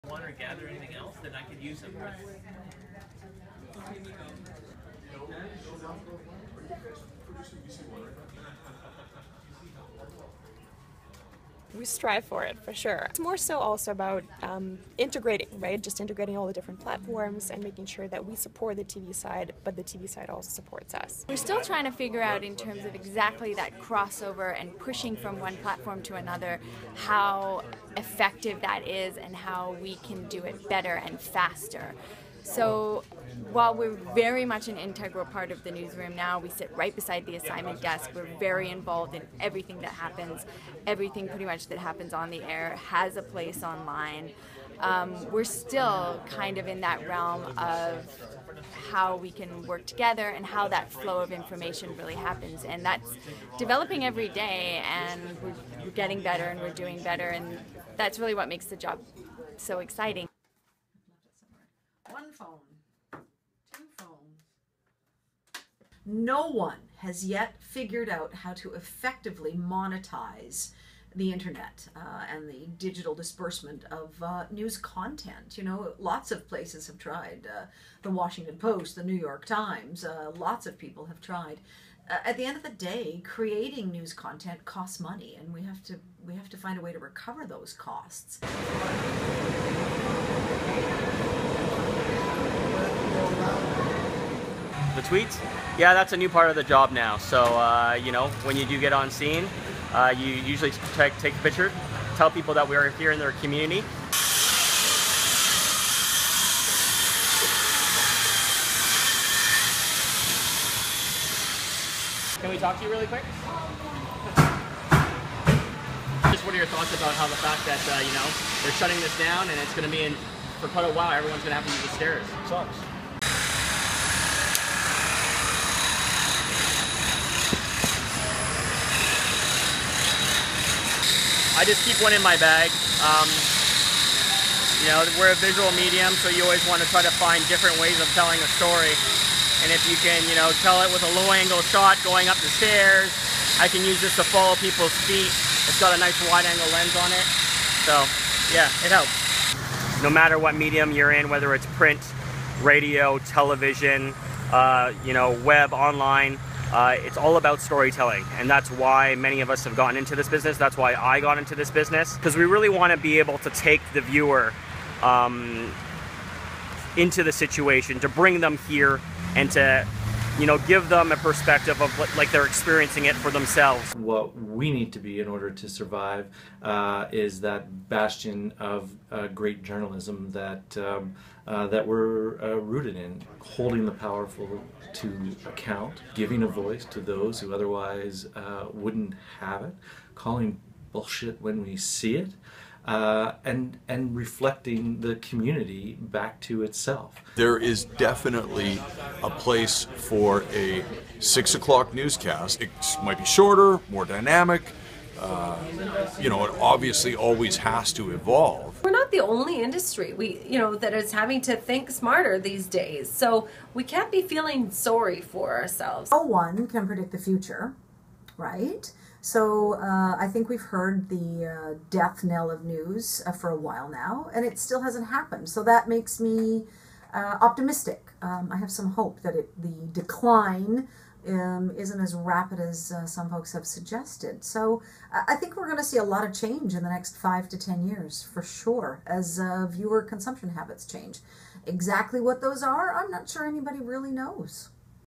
Global and the Digital News Revolution
Like most news operations, Global BC is facing a challenge with the changing nature of how people are getting their information these days. BCIT Journalism's Bill Amos spent some time there and has this look at how Global is approaching the future.